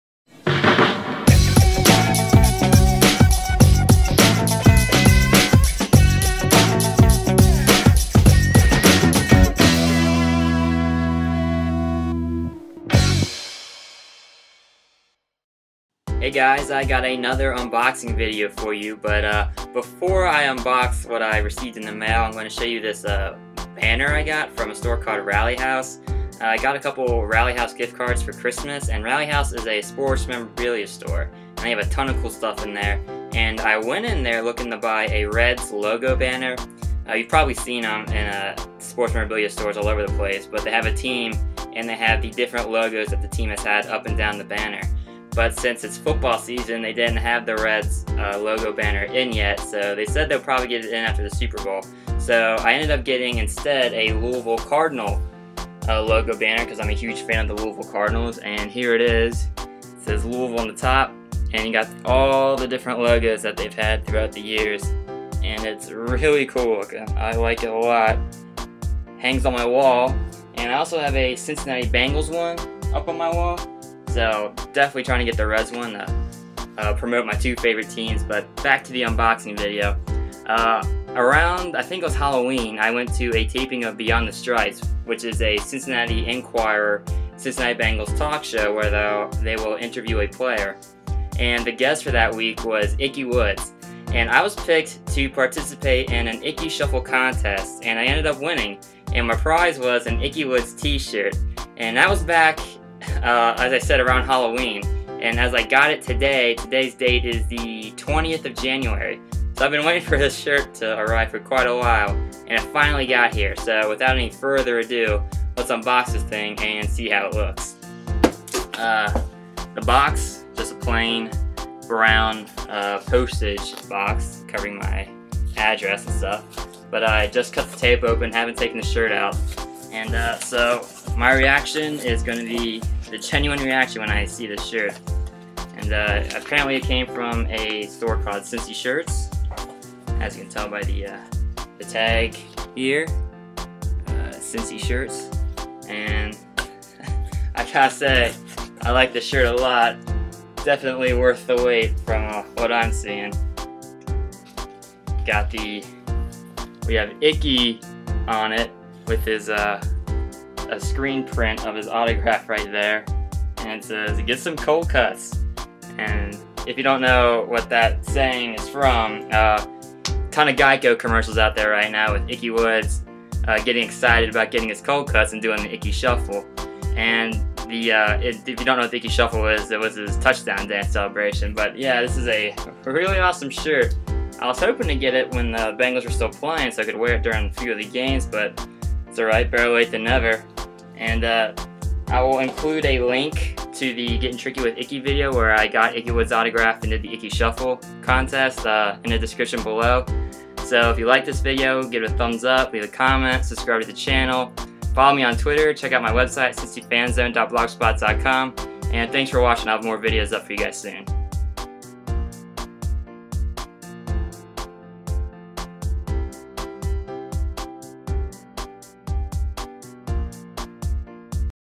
Ickey Woods Cold Cuts Shirt Unboxing
Ryan of Cincy Fan Zone unboxes the Ickey Woods Cold Cuts shirt he won at Beyond the Stripes.

Check out the Cincy Fan Zone website at cincyfanzone.blogspot.com!